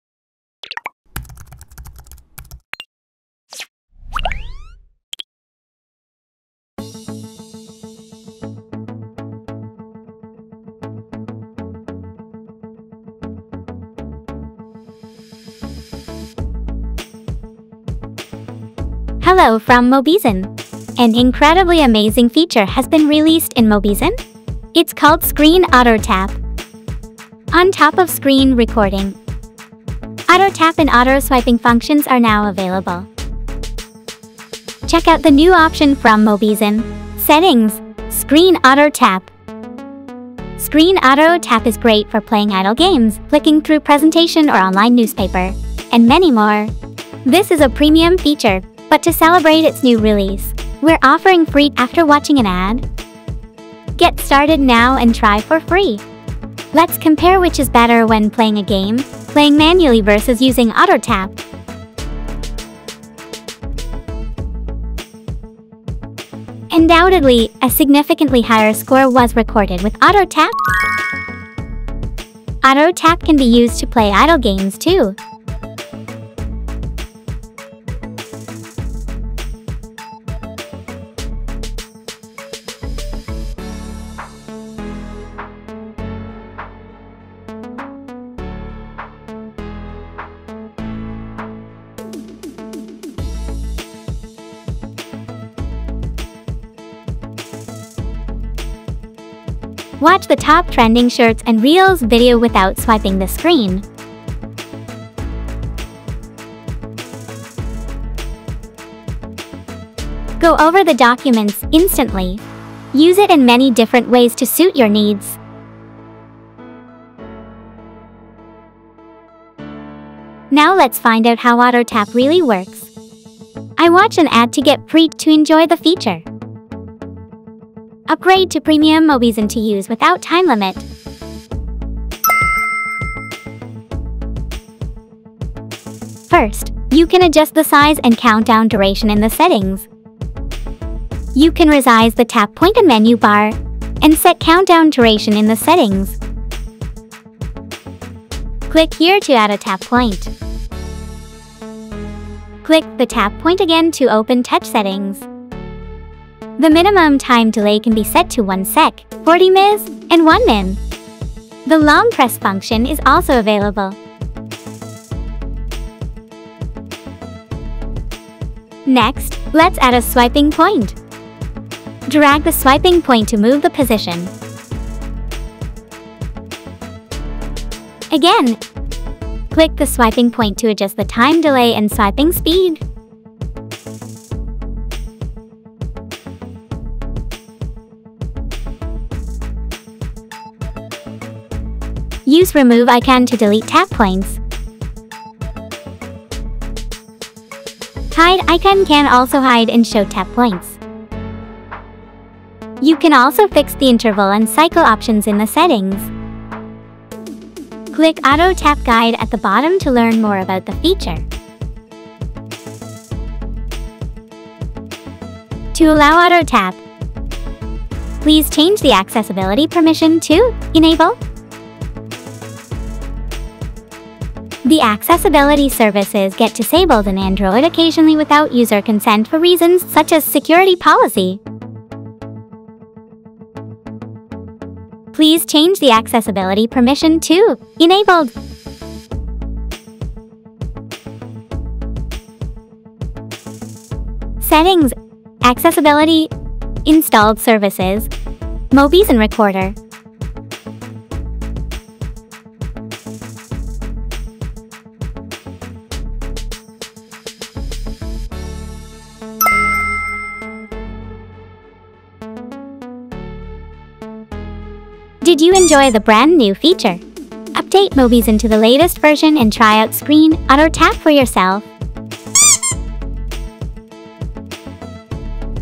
Mobizen Tutorial #4. Auto Touch
★ Auto-Touch feature is available in OS 7 and later ★
Provide Auto Tap and Auto Swiping functions
Meet Auto Tap and screen recording on Mobizen!

Multi-language subtitle (Supported language)
- 한국어
- عربى
- português
- Español
- русский
- bahasa Indonesia
- English
- Türk
- ไทย
- Deutsche
- ភាសាខ្មែរ
- français
- italiano
- Tiếng Việt
- বাংলা
- Polskie
- فارسي
- 简体中文）
-日本語